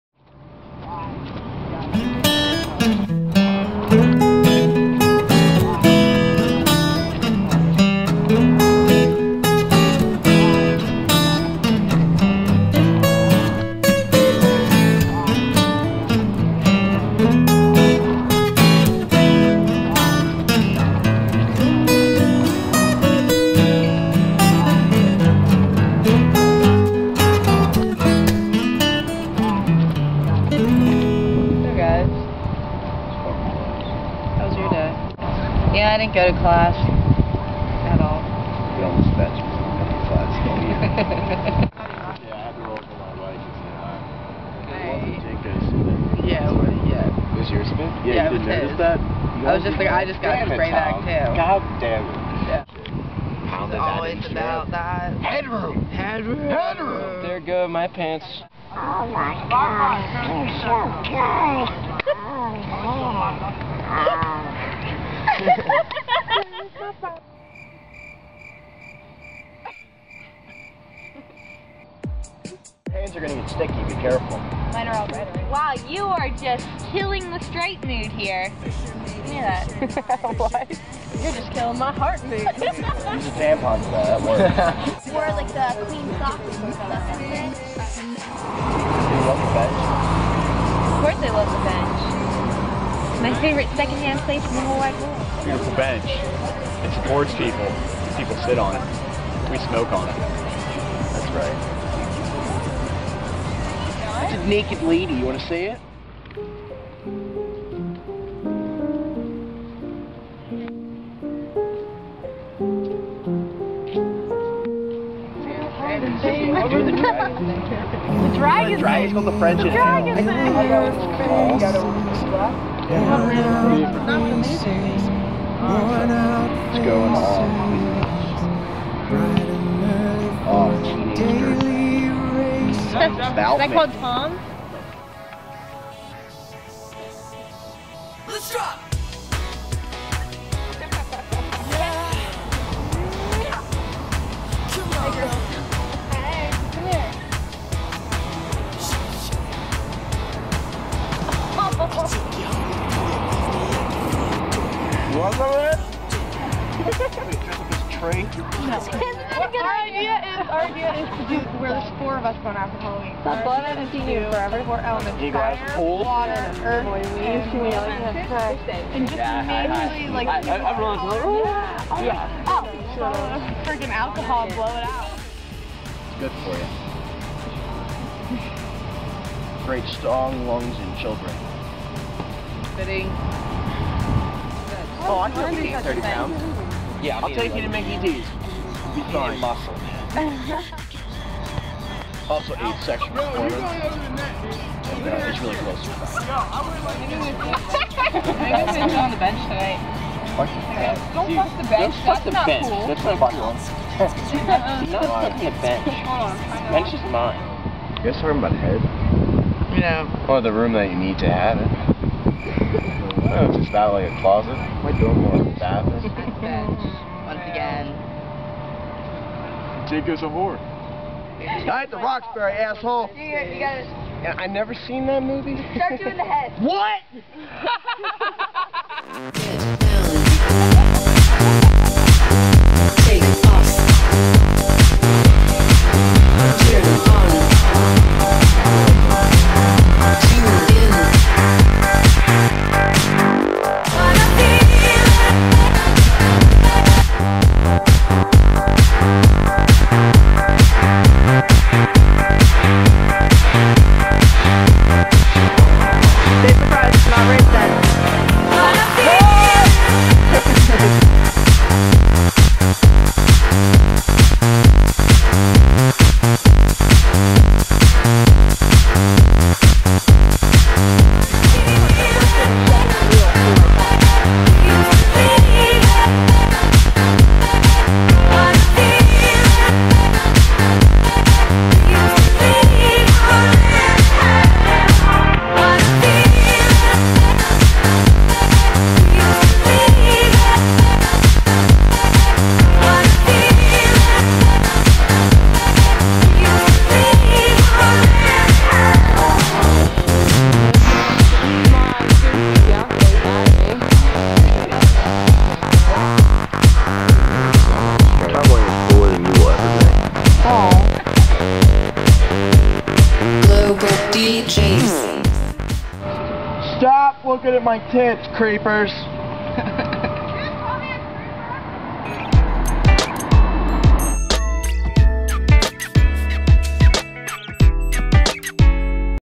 The Bench
A short film created to get the point across that this bench is the social center of West Side at Shippensburg University.